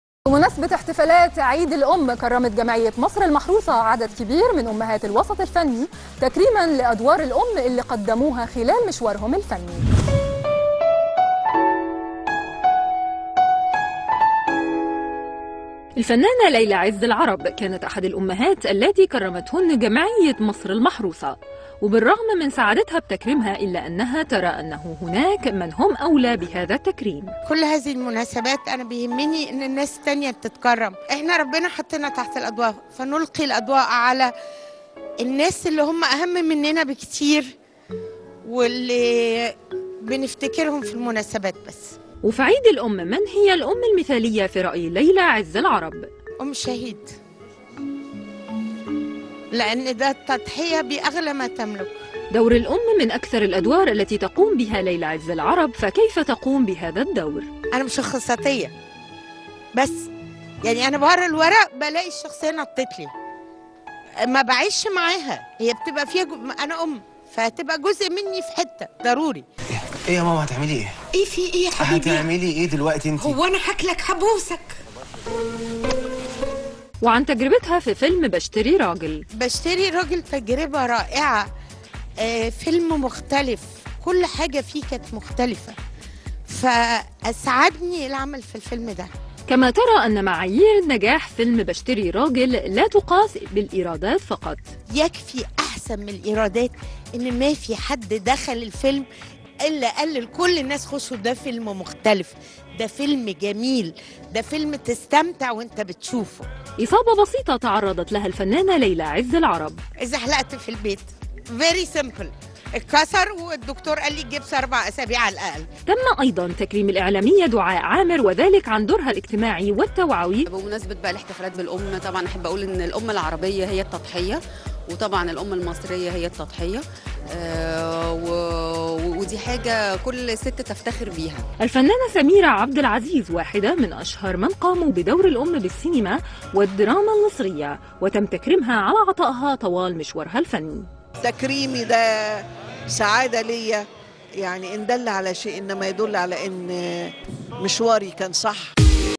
عرب وود l من إحتفالية وتكريم "جمعية مصر المحروسة" لنجمات الفن عن أدوار "الأم"
عرب وود l من إحتفالية وتكريم "جمعية مصر المحروسة" لنجمات الفن عن أدوار "الأم"  في "عيد الأم"